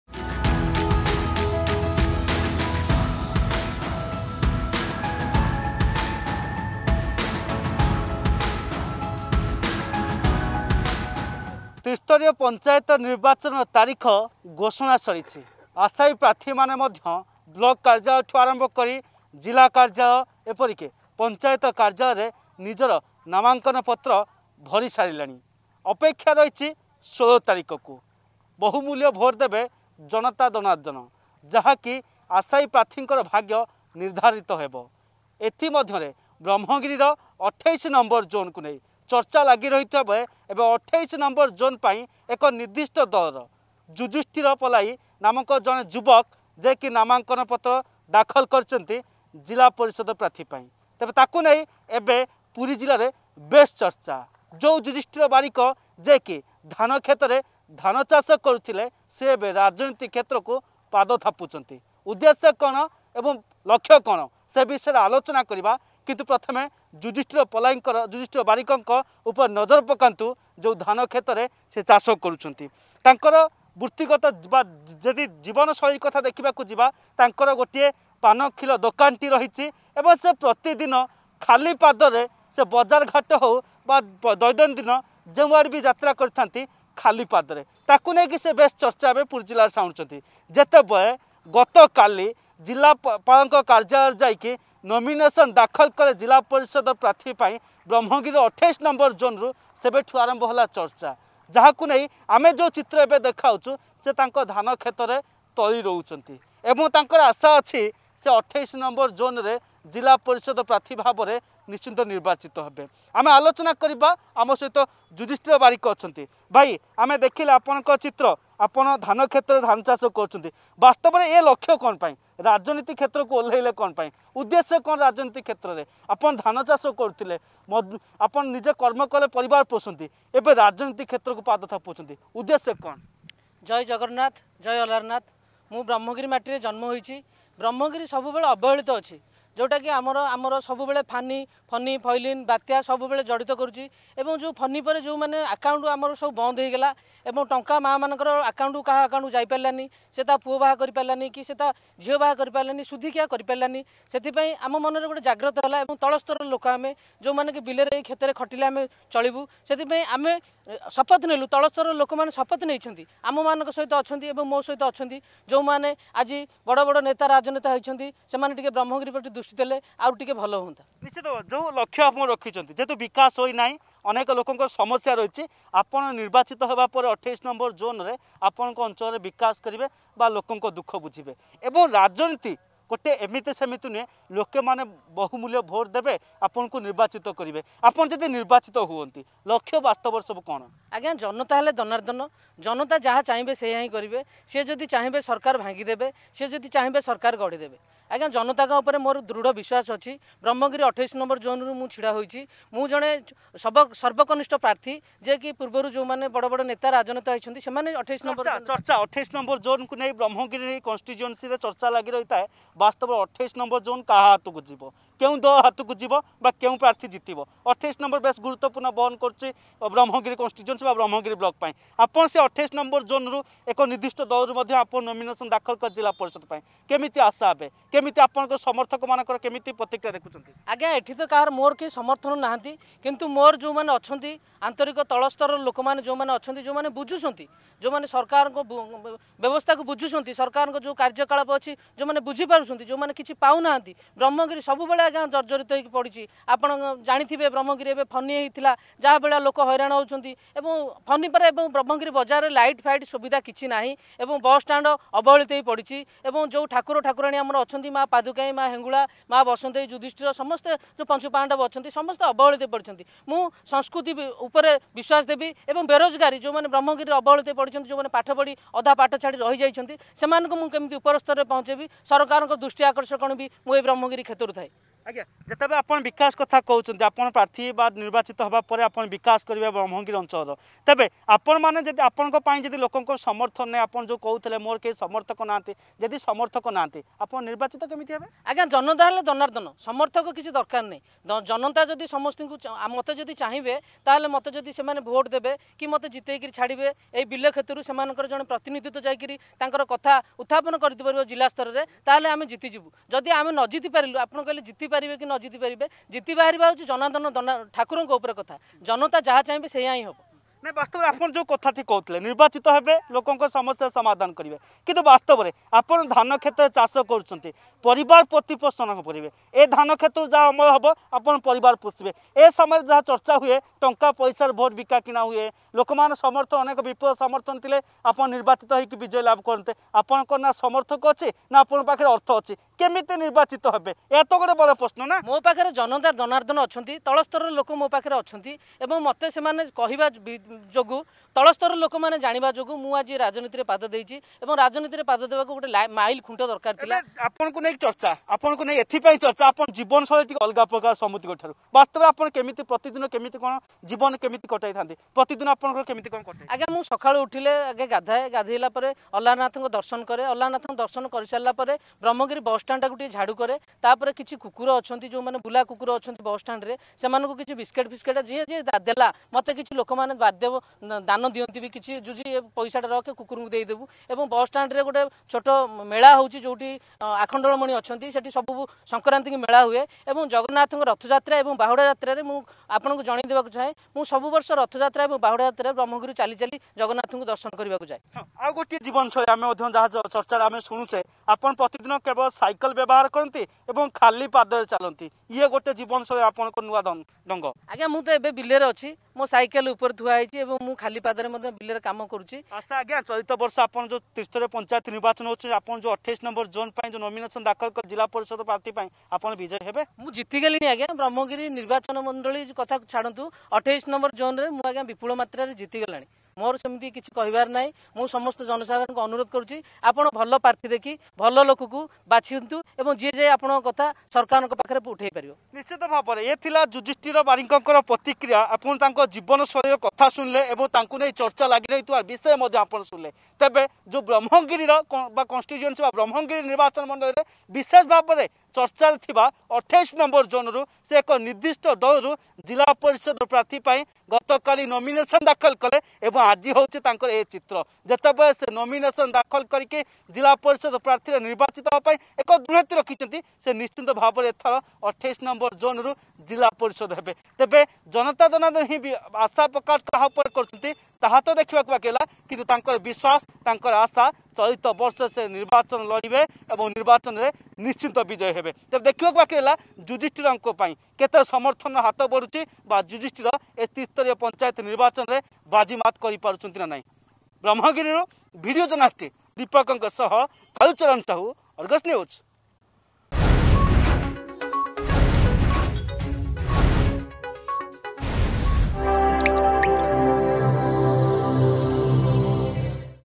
ଧାନ କ୍ଷେତରୁ ରାଜନୀତି ମୈଦାନ
ଧାନ କ୍ଷେତରୁ ରାଜନିତି ମୈଦାନକୁ ଓହ୍ଲାଇଛନ୍ତି ଯୁଧିଷ୍ଟିର ବାରିକ । ବ୍ରହ୍ମଗିରି ବ୍ଲକ ୨୮ନଂ ଜୋନ ଜିଲ୍ଲାପରିଷଦ ପାଇଁ ନିର୍ଦ୍ଧିଷ୍ଟ ଦଳର ପ୍ରାର୍ଥୀ ଭାବରେ ନାମାଙ୍କନ ଭରିଲେ ଯୁଧିଷ୍ଟିର